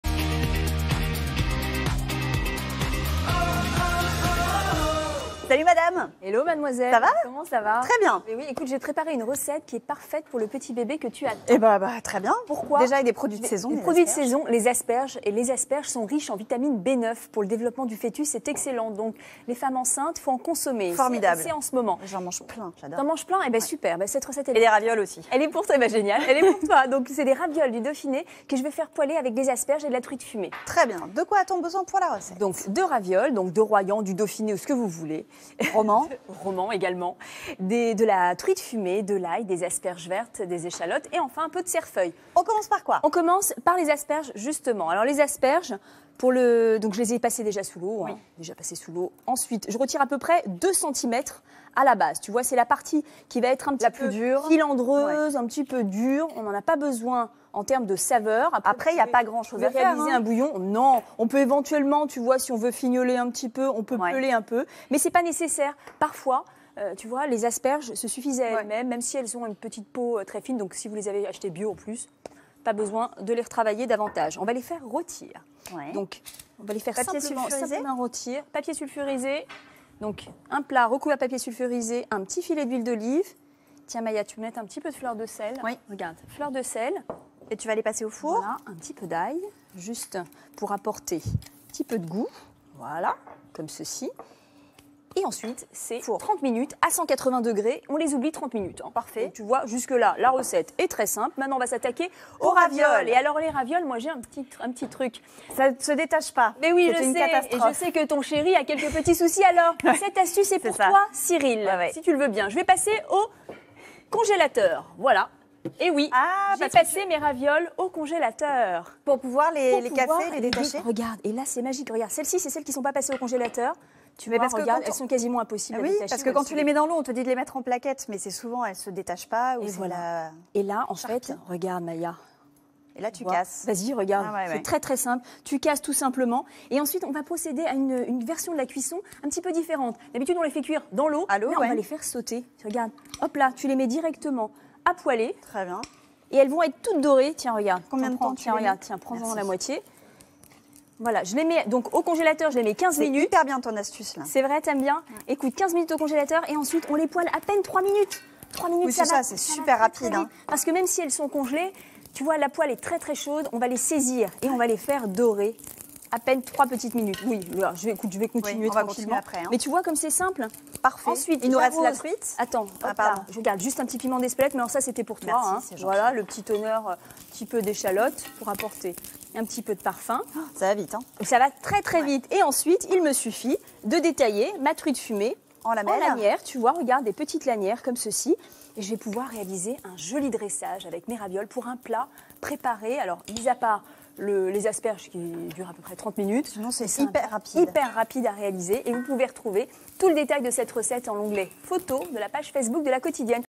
Coup d'food : Ravioles à la truite fumée et asperges rôties
Aujourd'hui, Farida nous prépare des ravioles à la truite fumée et asperges rôties.